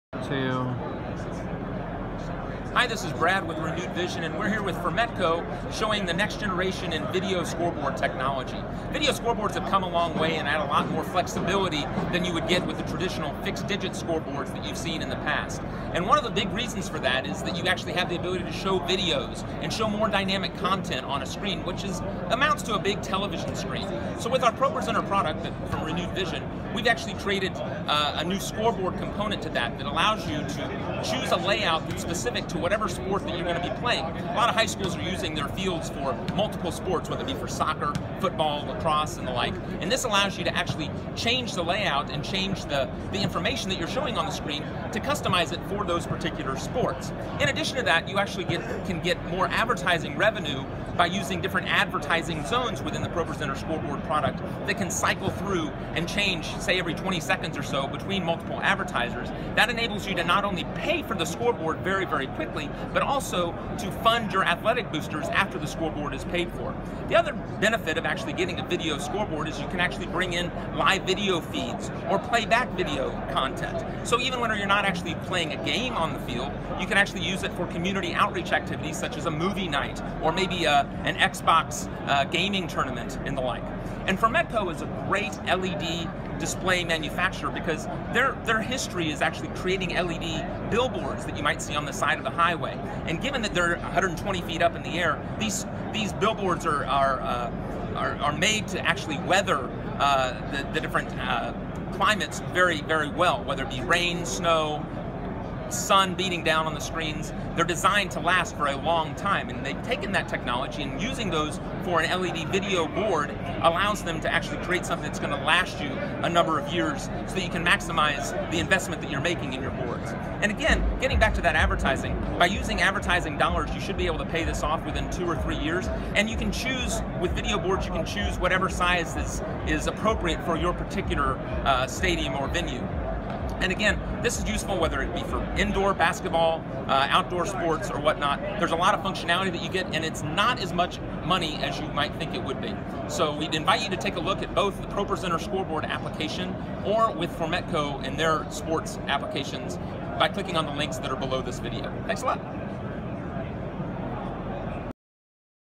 Formetco Sports / ProPresenter LED Scoreboard Overview
Formetco offers a wide range of Multi-Purpose LED Scoreboards and LED Video Entertainment Systems. We support football, baseball, lacrosse, soccer, and track with our scoring software. With the use of our innovative technology we offer a large LED screen designed to increase your attendance, and increase the number of events you can host at your facility.

Renewed Vision's easy to use software gives you the ability maximize the capabilities of a high impacted video screen for community events, charity events, band competitions, graduations, movie night, and food truck events. You name it and we can make the event more exciting for the attendees.